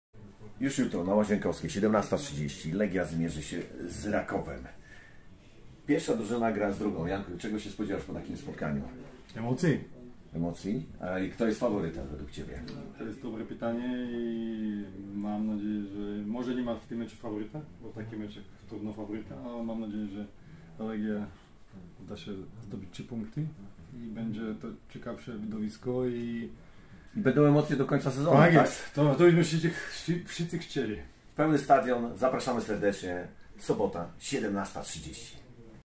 Byli piłkarze Legii o szansach na wygraną z Rakowem.
Tylko w Radiu dla Ciebie Tomasz Sokołowski i Jan Mucha typują faworyta meczy Legia Warszawa vs. Raków Częstochowa.

🎙️ Zaprasza Agnieszka Kobus-Zawojska i Łukasz Starowieyski
📅 poniedziałek 🕛 godz. 09:00

🎙️Zaprasza Arkadiusz Błaszczyk i Tomasz Sokołowski
📅 piątek 🕛 godz. 09:00